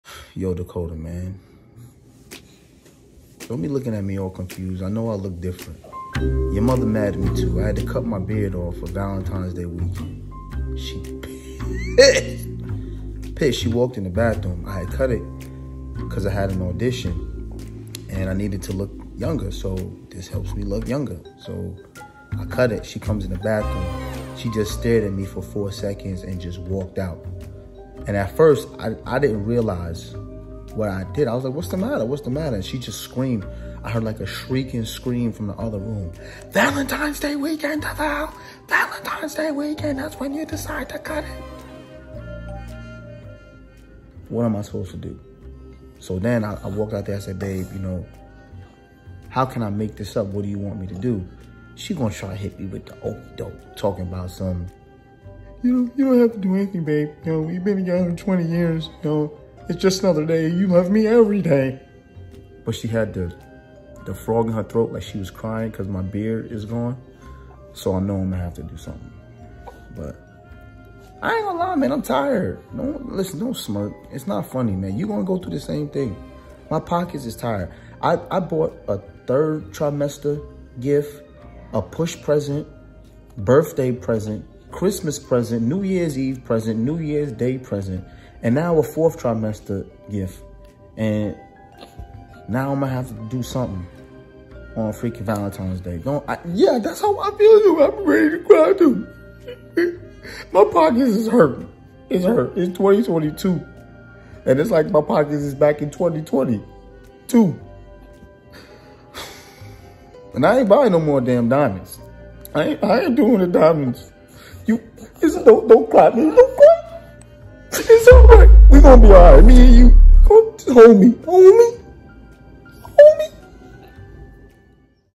Valentine’s Day and No Beard
Kota Konversations: Kay Kay mad I had to cut my beard on Valentines Day weekend.🤷🏾‍♂️ Now I gotta go buy something to distract her till it grows back.🙄 You guys have no idea how hard it is to be a man with a beard these days.😔 I feel so used at times for her sexual exploits, but I guess that’s the sacrifice I have to make in this husband life.😏 DEAD ASS!